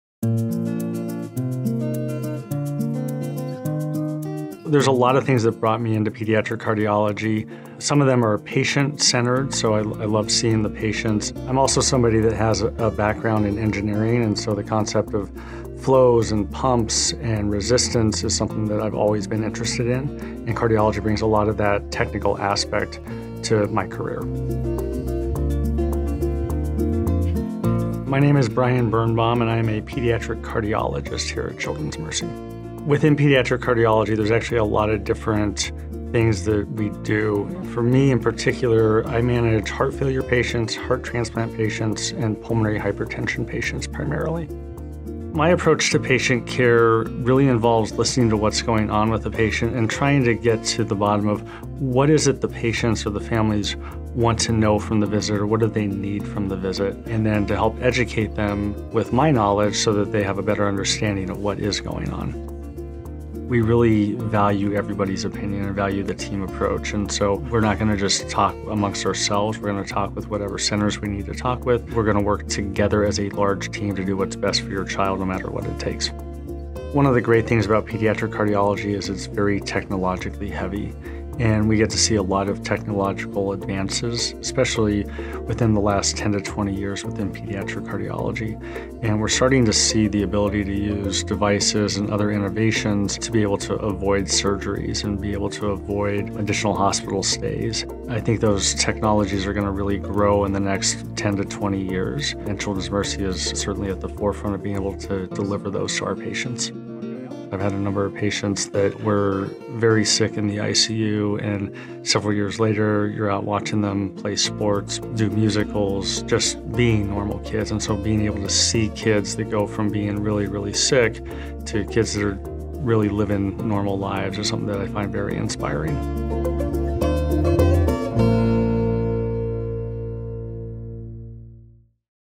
Meet the Experts: Brian Birnbaum, MD - Pediatric Cardiology
Meet Brian Birnbaum, MD, Director of Pulmonary Hypertension Program and Medical Director of the Heart Failure Program at The Ward Family Heart Center at Children’s Mercy Kansas City.

Dr. Birnbaum is proud to be the medical director of the Heart Failure Program at Children’s Mercy. This team combines experts from several departments of the hospital to provide care for children and teens who are in various stages of heart failure, preparing for heart transplant or who have undergone heart transplant. To learn more about Dr. Birnbaum and the Heart Failure Program,